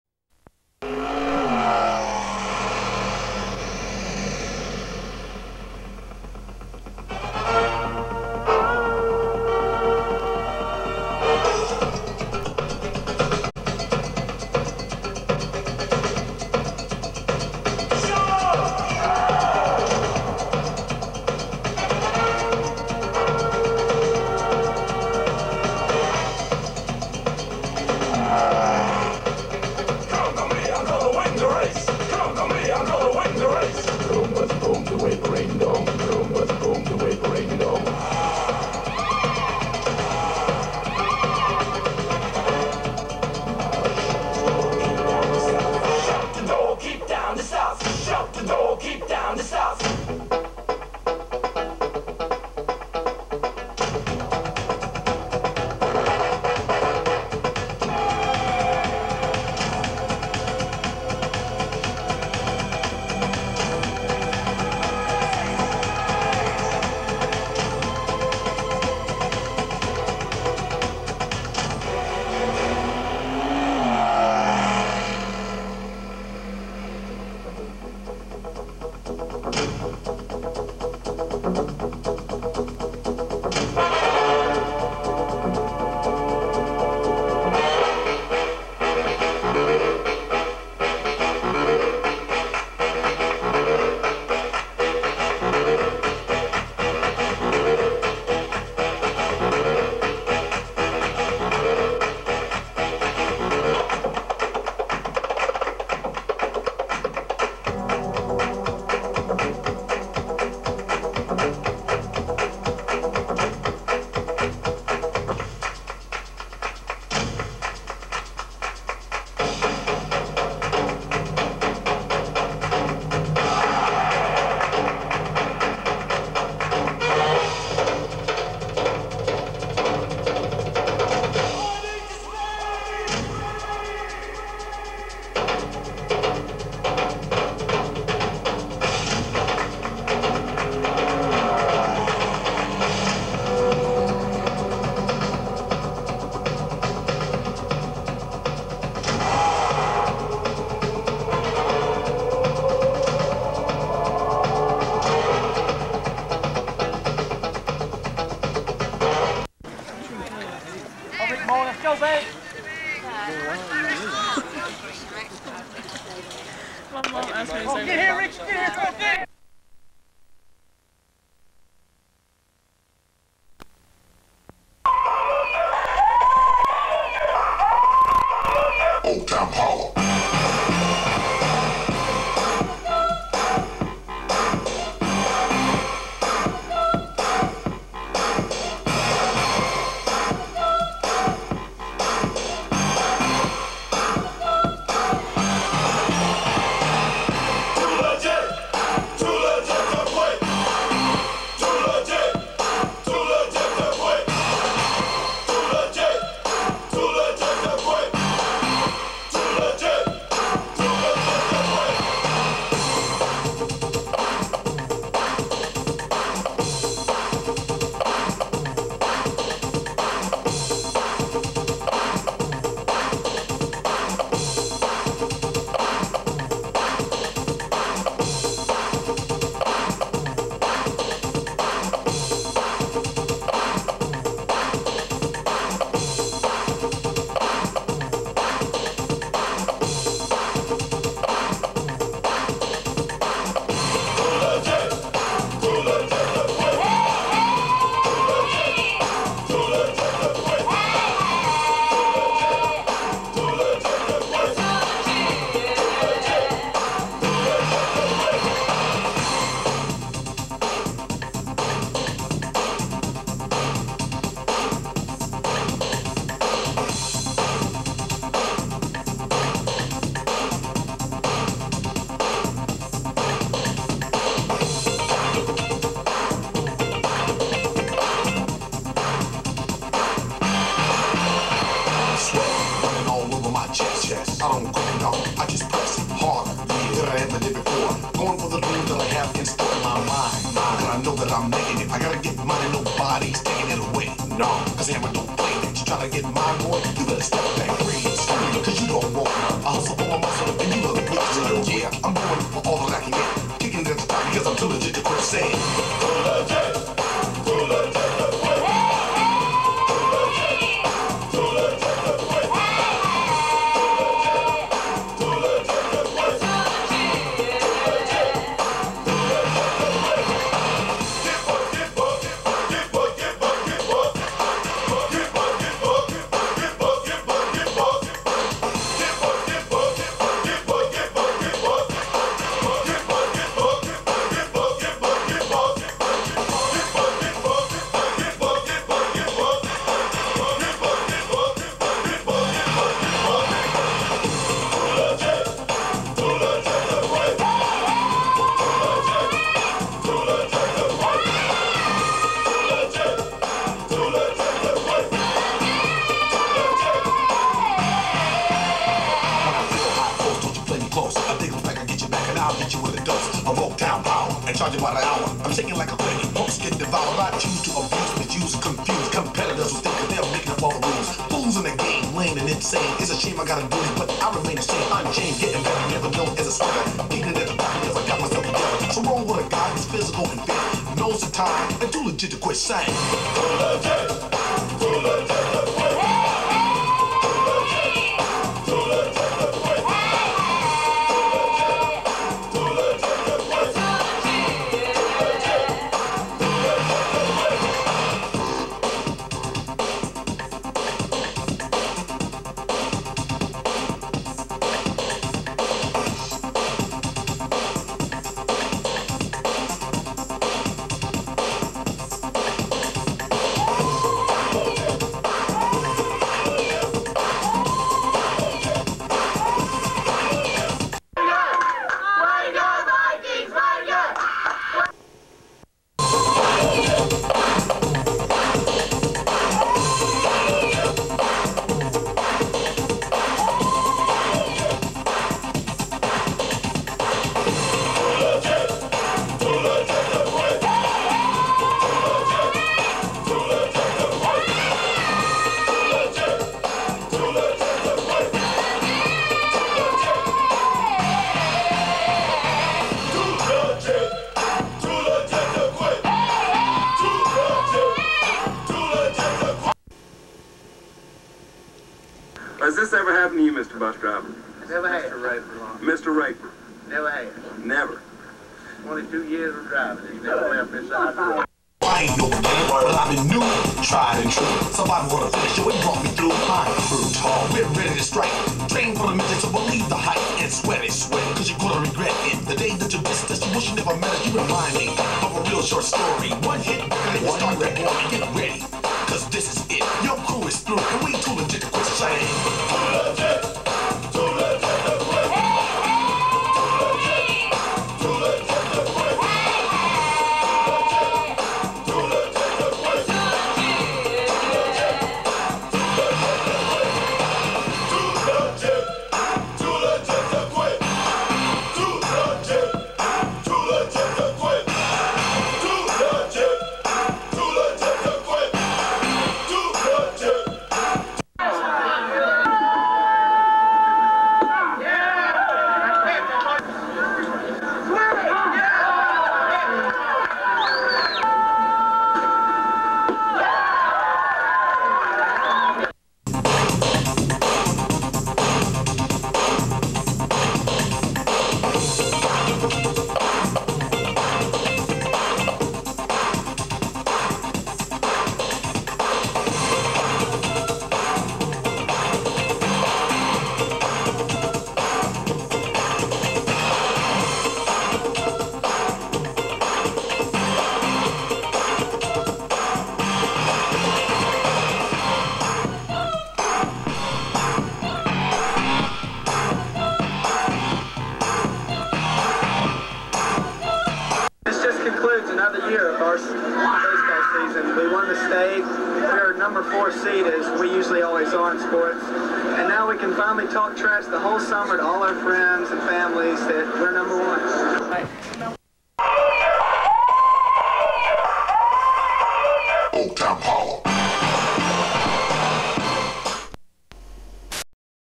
1992 First Presbyterian Day School Baseball State Championship Highlight video
This is a video montage of some of the highlights from FPD's 1992 state championship in baseball.